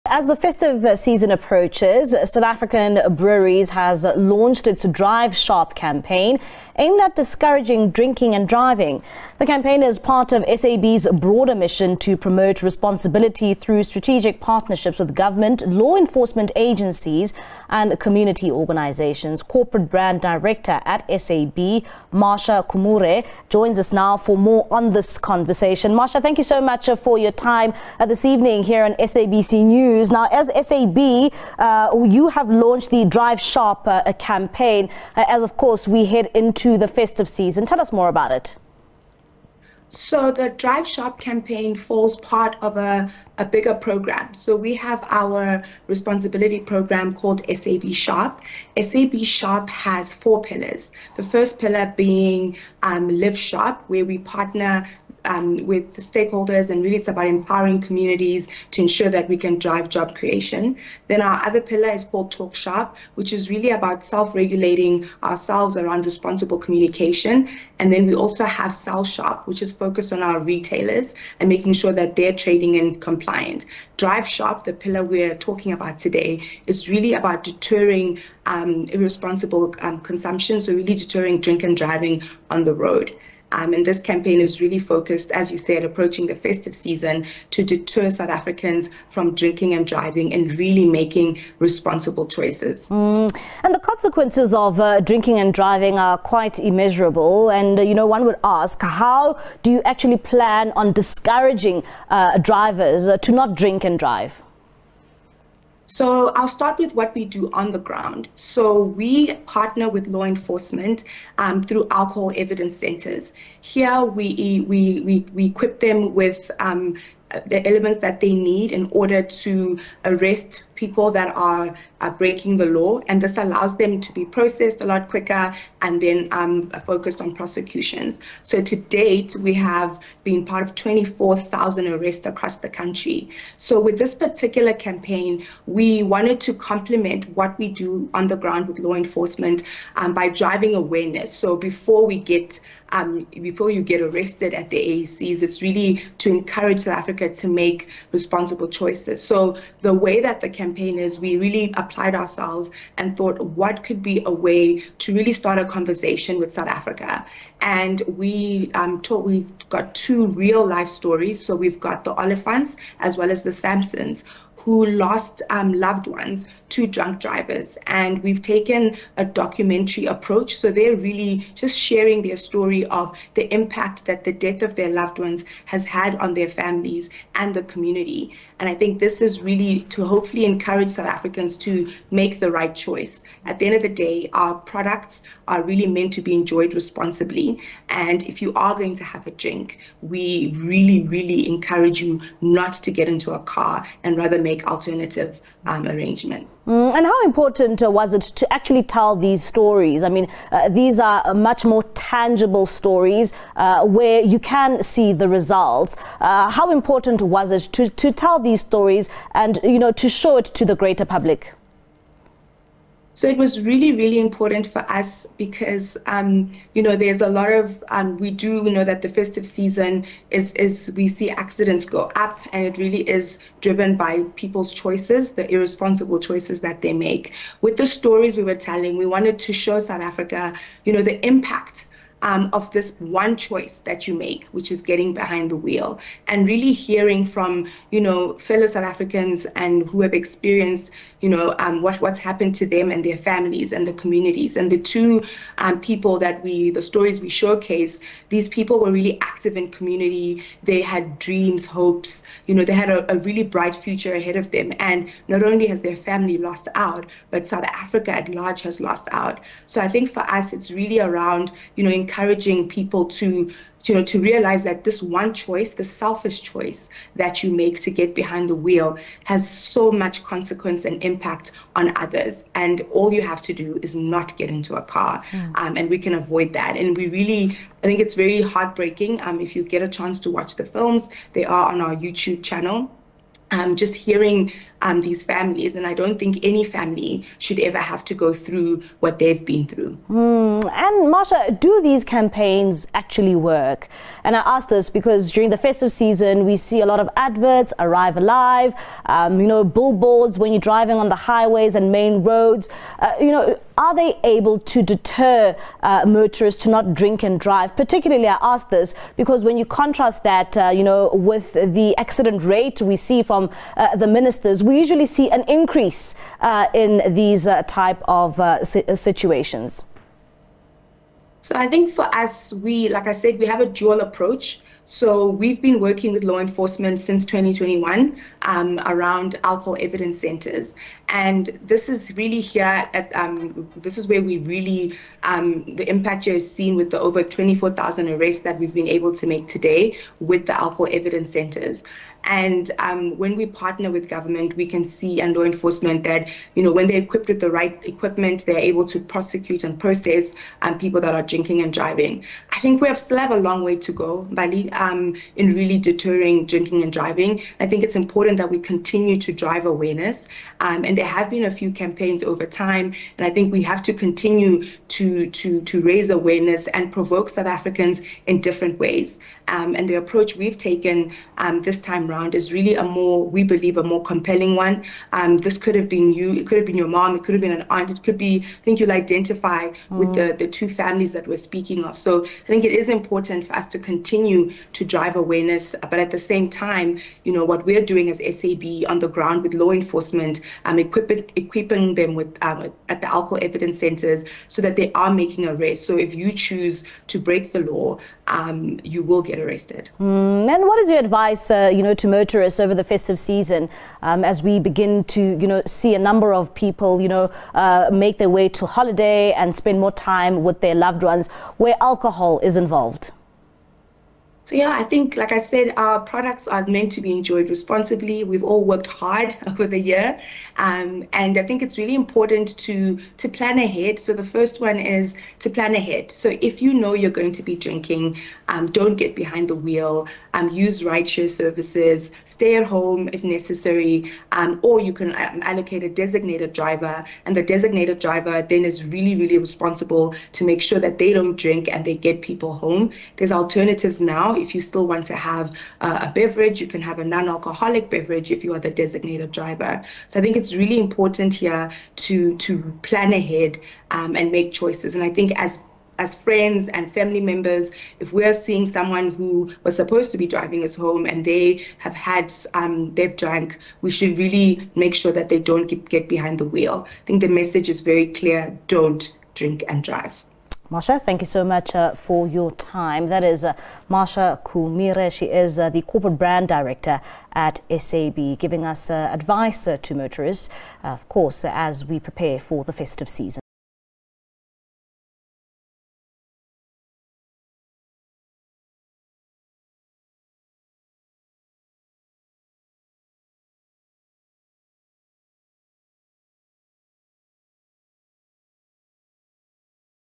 SAB's festive campaign aims to discourage drinking and driving
As the festive approaches, South African Breweries has launched its Drive Sharp campaign, aimed at discouraging drinking and driving.

The campaign is part of SAB's broader mission to promote responsibility through strategic partnerships with government, law enforcement agencies, and community organisations.

Corporate brand director at SAB, Marsha Kumire, joins us for more.